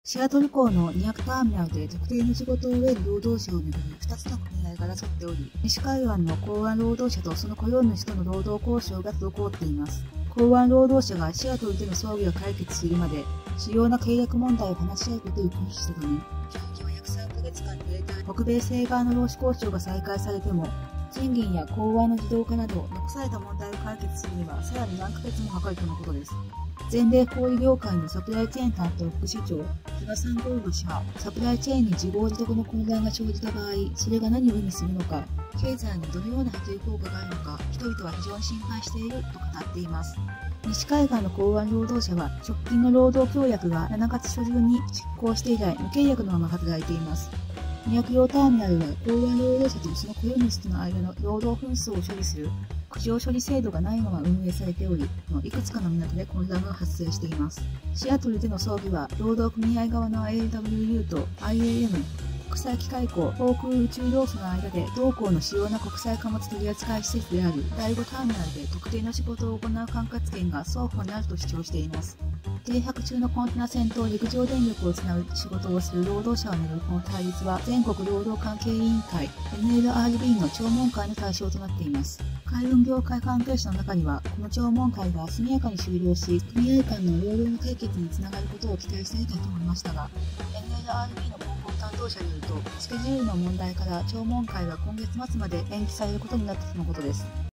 シアトル港を巡る２つの組合の争いで北米西岸労使交渉停滞
ロングバージョンはこちらから